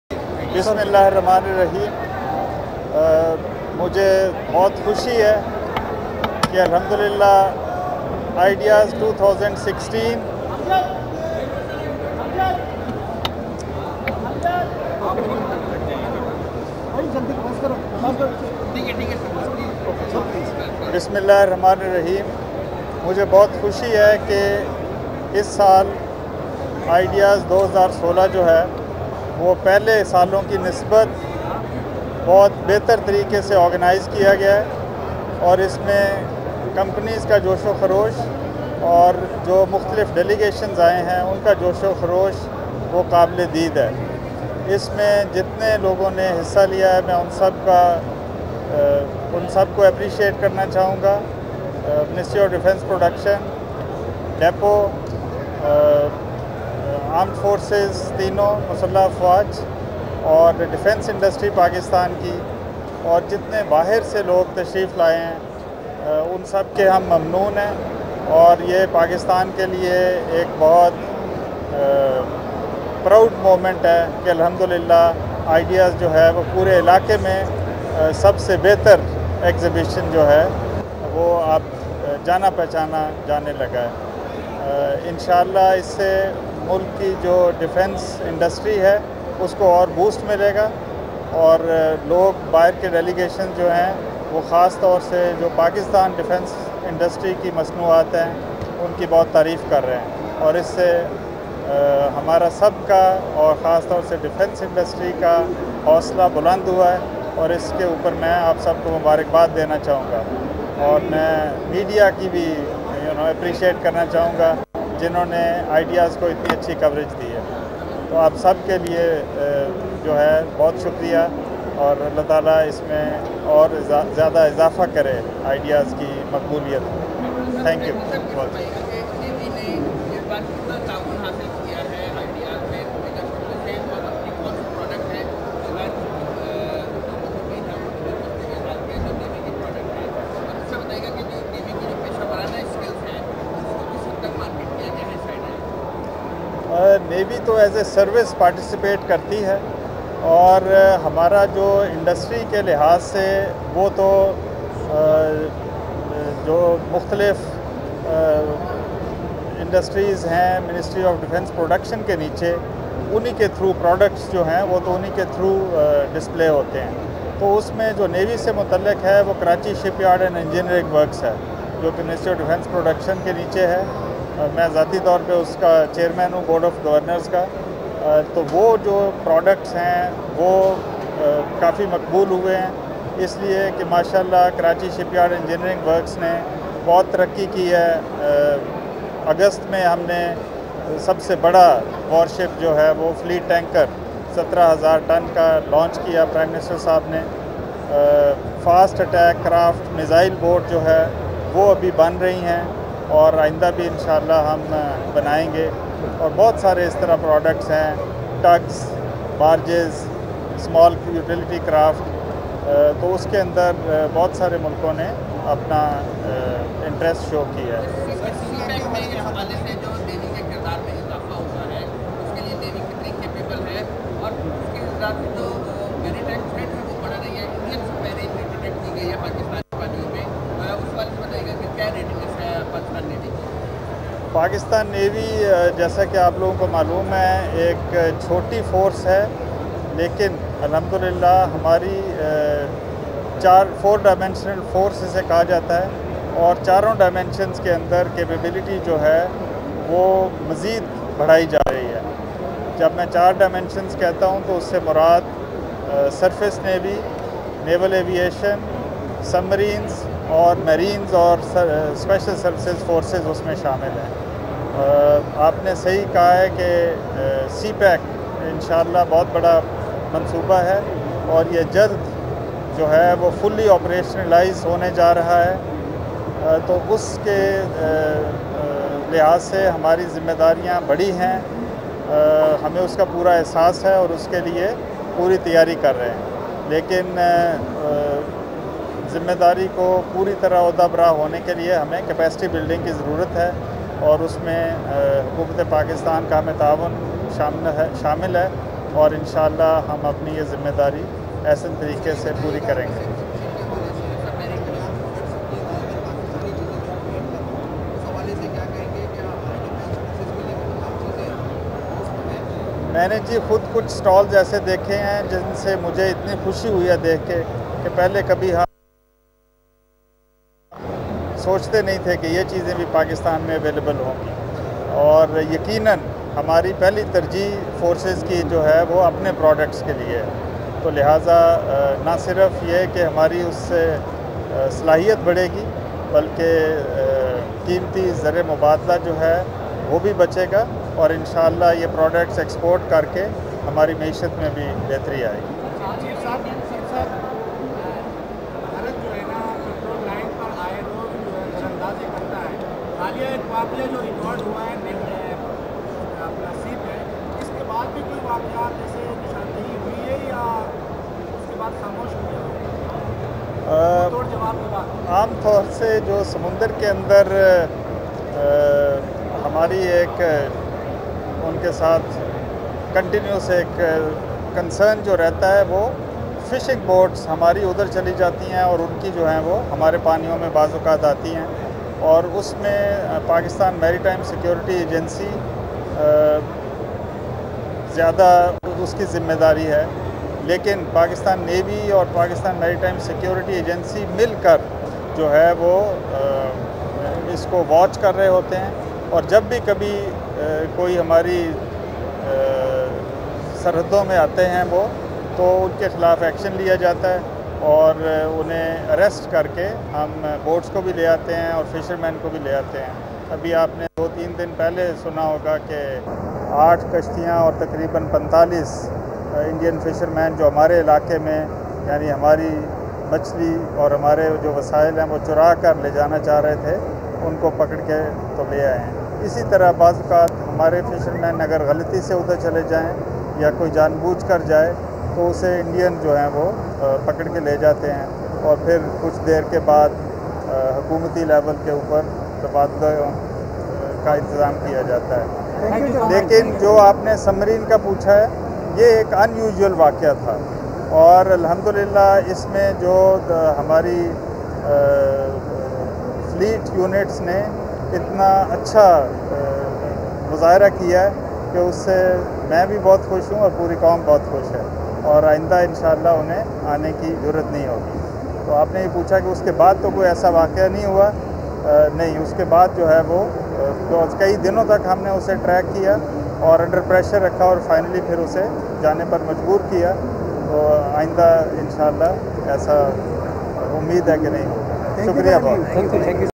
Chief of Naval Staff Admiral Muhammad Zakaullah Talk 24 November 2016
Chief of the Naval Staff Admiral Muhammad Zakaullah Talk 24 November 2016